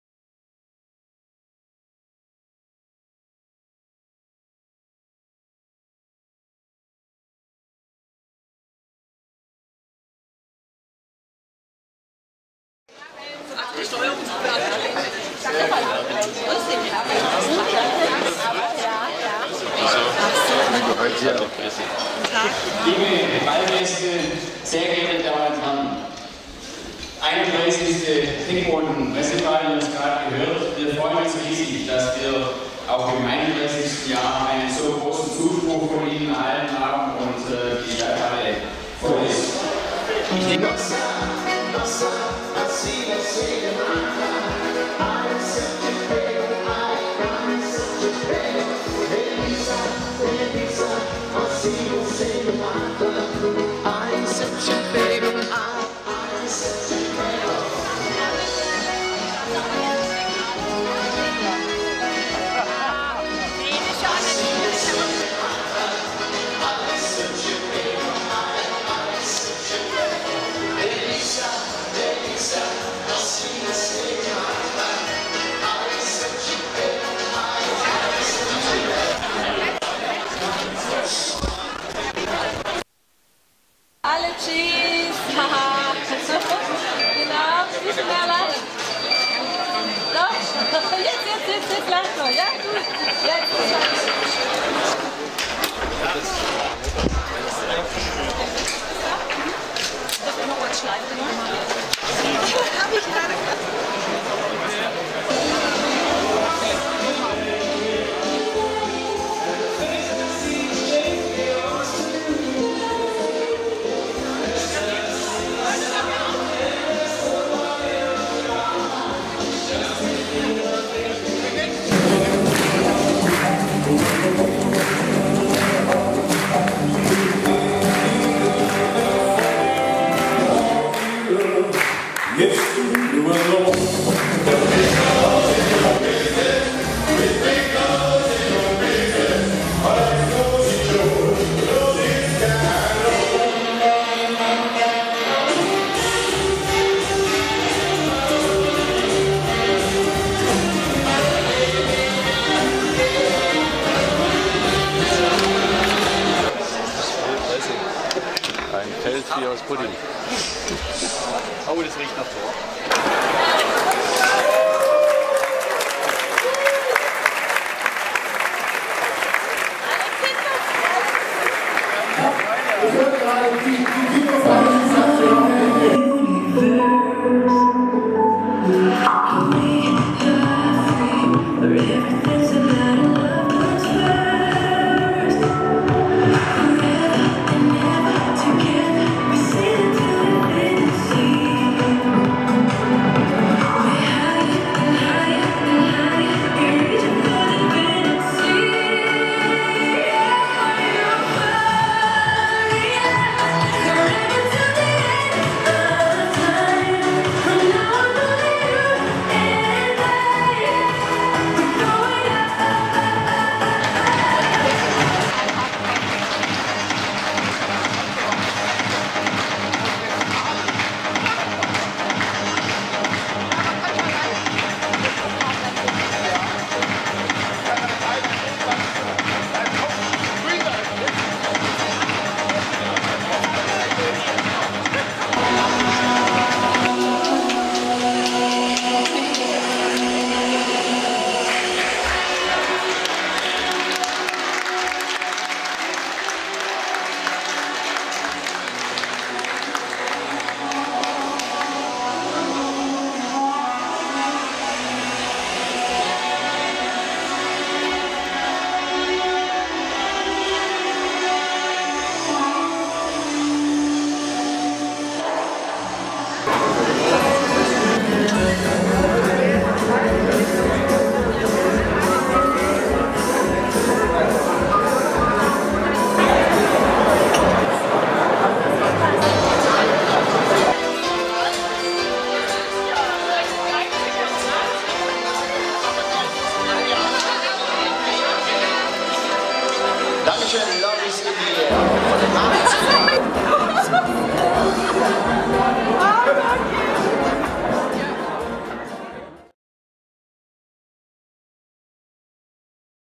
31. Presseball des Teckboten
Eine Tanznacht mit besonderen modischen Akzenten hat der Teckboten-Presseball in der Stadthalle dem Kirchheimer Publikum beschert: Vom Petticoat bis zum Zeitungskleid reichte die Bandbreite dessen, was die Damen bei den Show-Einlagen präsentierten, am Samstag,18.Oktober,in der Kirchheimer Stadthalle.